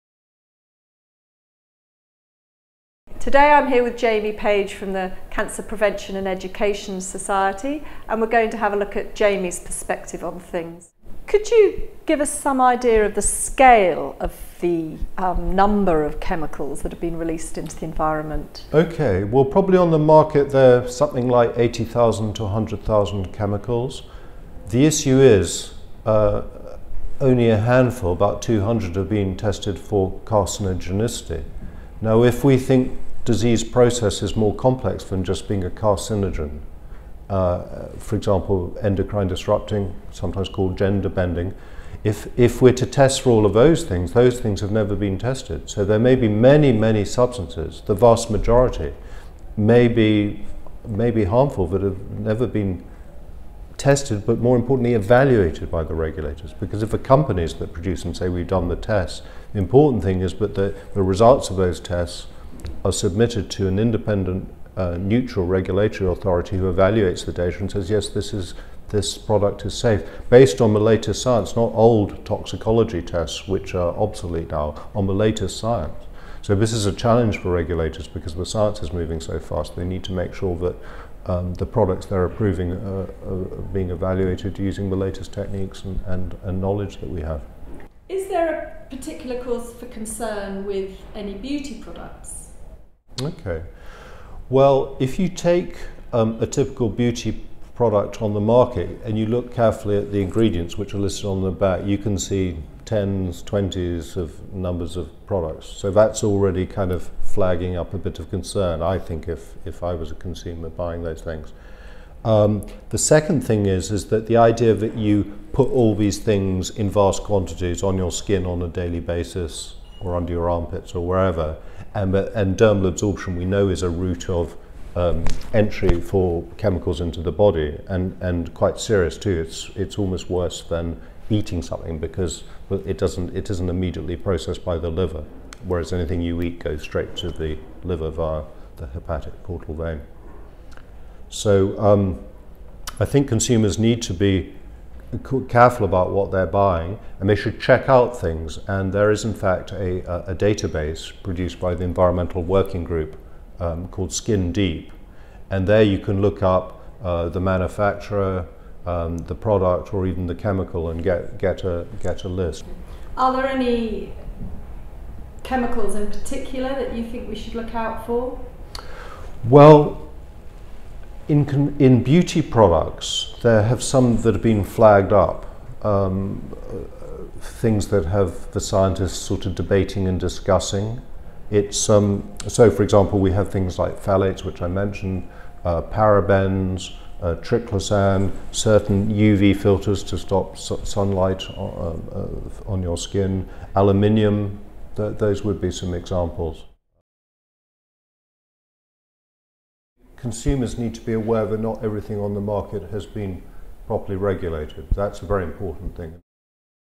The health risks of chemical beauty
NYR Natural News' Susan Curtis talks to Jamie Page, Chief Executive of the Cancer Prevention Society, about the chemcials commonly found in cosmetics and their influence on health. For more about natural health and medicine